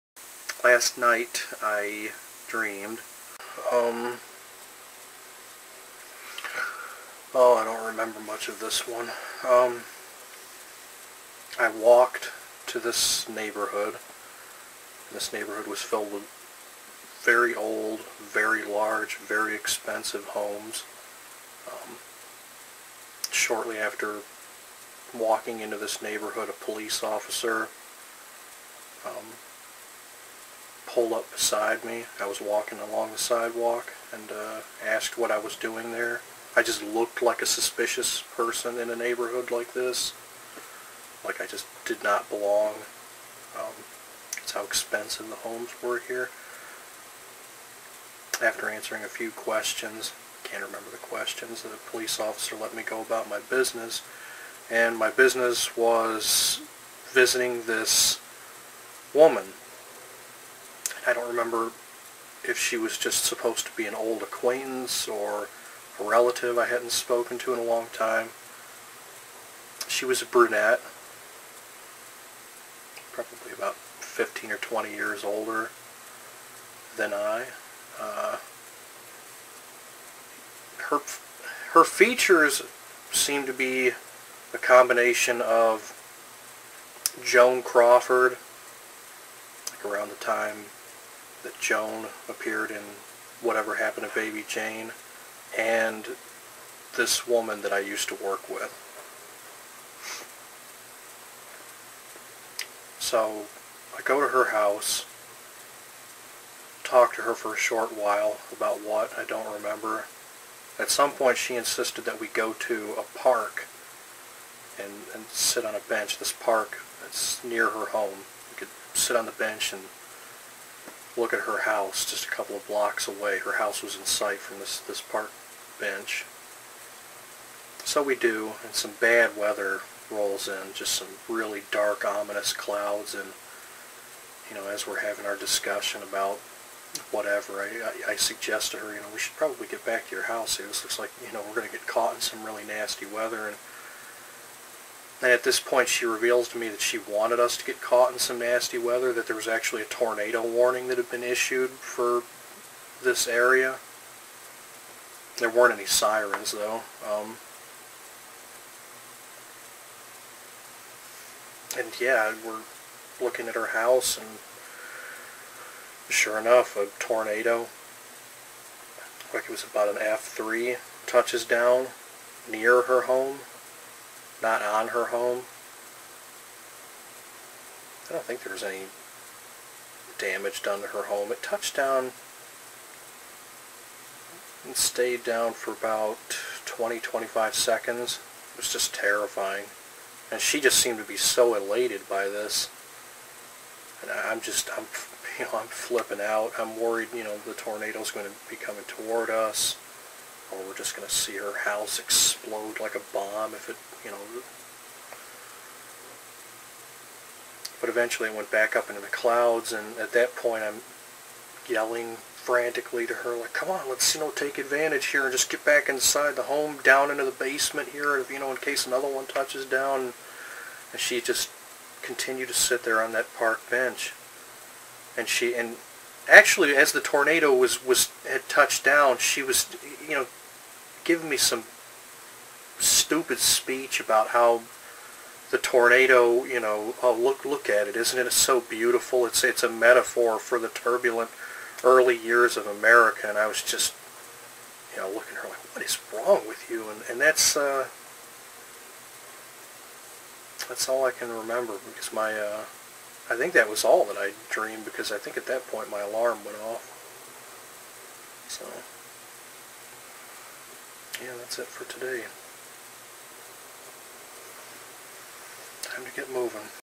Joan Crawford And I Watch A Tornado Destroy Her Home
Last Night I Dreamed 7: Joan Crawford And I Watch A Tornado Destroy Her Home

Frightening events described in this, the one week anniversary episode(!):
* Walking into a very nice neighborhood with lots of old, large, beautiful, expensive homes.
* Being questioned by local police just for looking like a suspicious person.
* Having a conversation on a park bench with a woman who resembled Joan Crawford just as a Tornado touched down near us.
* Being lectured by the Joan Crawford lookalike on the subject of the early, "turbulent" years of America.

Why do we dream?  What do our dreams mean?  I'll leave that for you to decide in the comments below.  I'm just going to describe mine.